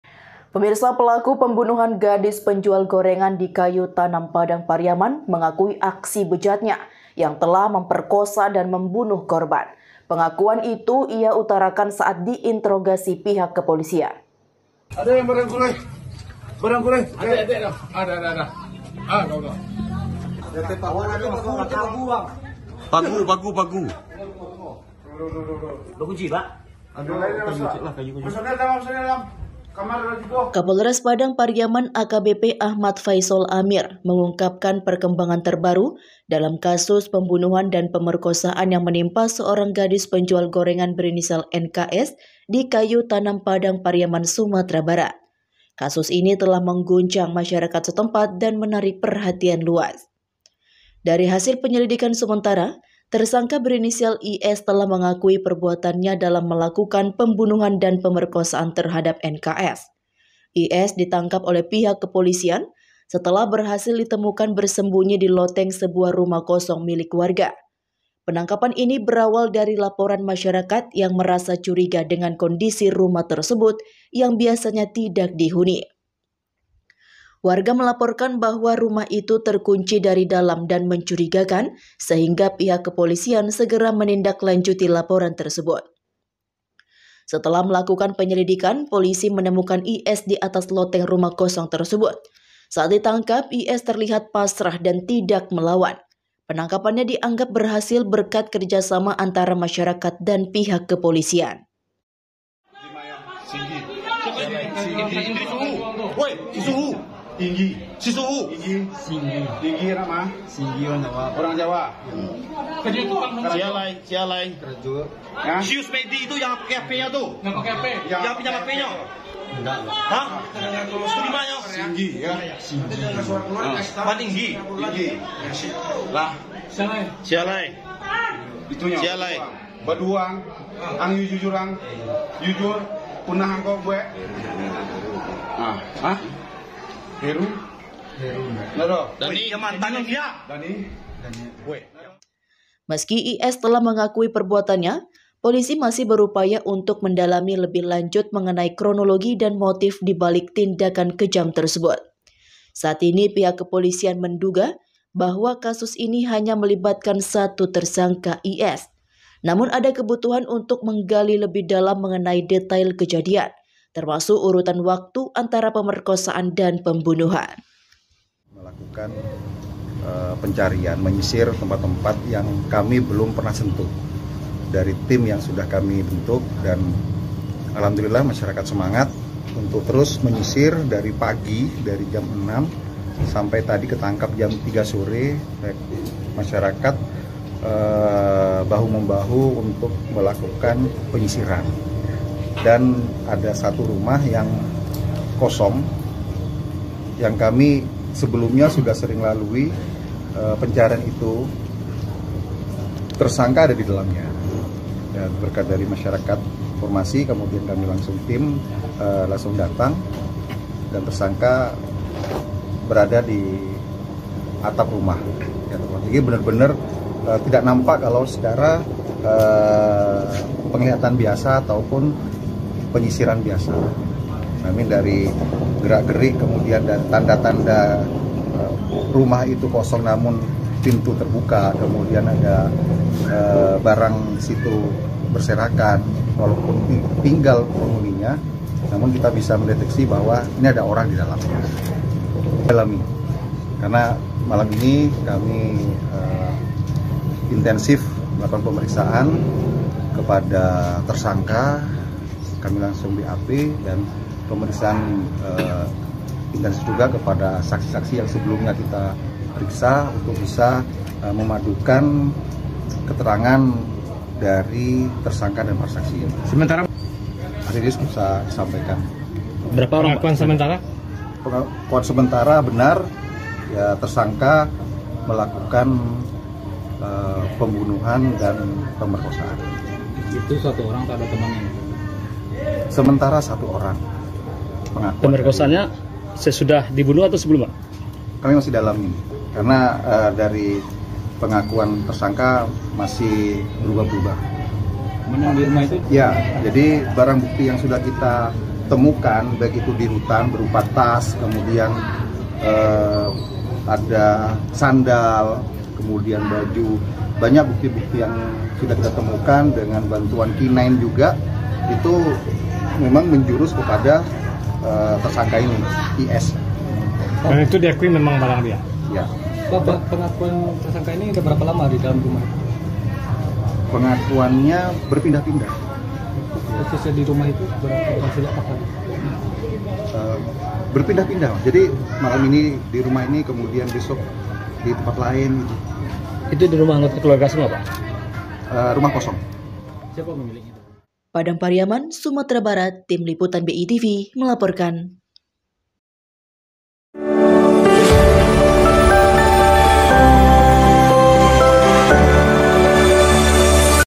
IS Akui M3mp3rk*s4 dan M3mb*n*h Gadis Penjual Gorengan di Padang Pariaman
P3l4k* p3mb*n*h4n gadis penjual gorengan di Kayu Tanam, Padang Pariaman mengakui aksi b3j4tnya yang telah m3mp3rk*s4 dan m3mb*n*h k*rb4n. Pengakuan itu ia utarakan saat diinterogasi pihak kepolisian.